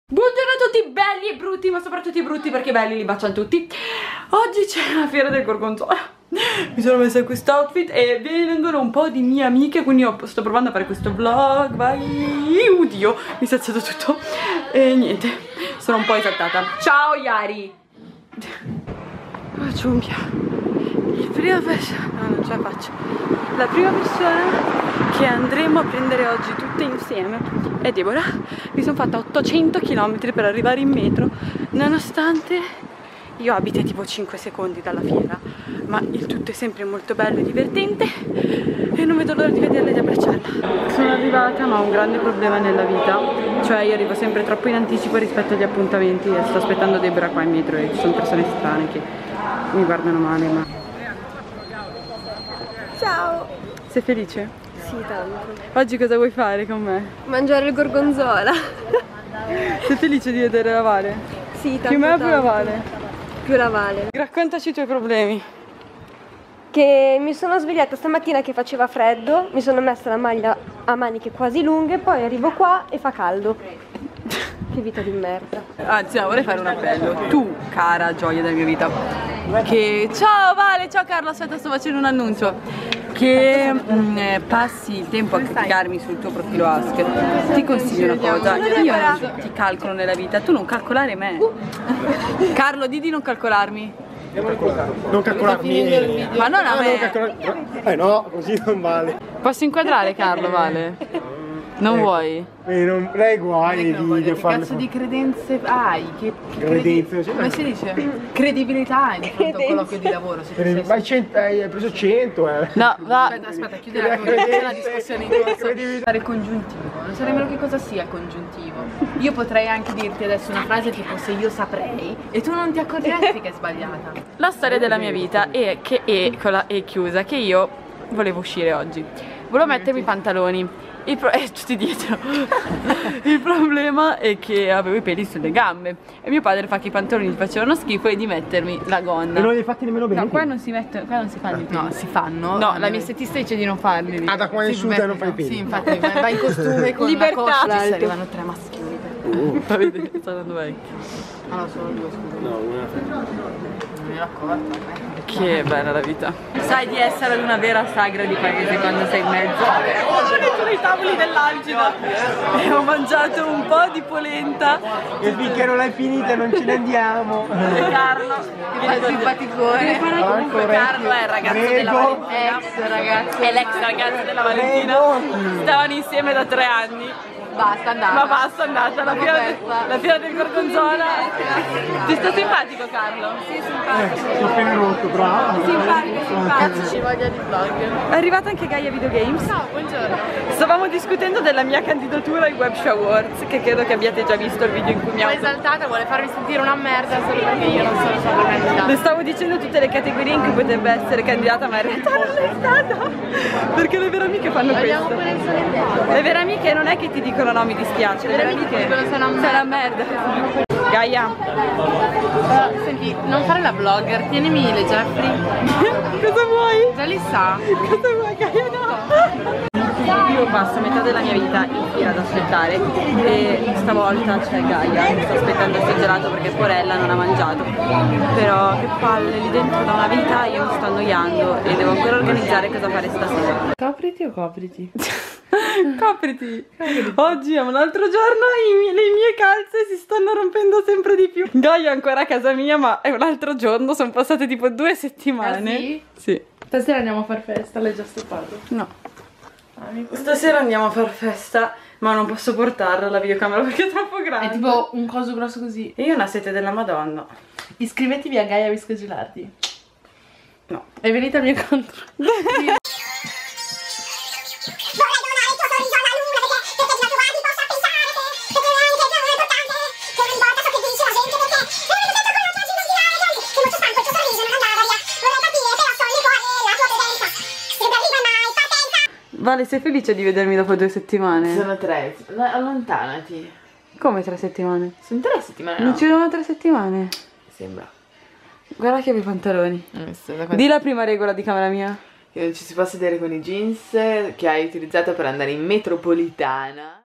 TRE MASCHIONI PER ME
CONTATTAMI/LEGGERE: PERCHÈ QUESTO VIDEO?
Periscope: jalired

Ho deciso di pubblicare questo video perché è il riassunto delle cazzate che combino in un mese e mezzo ormai.
Sono le parti più casalinghe e free della mia vita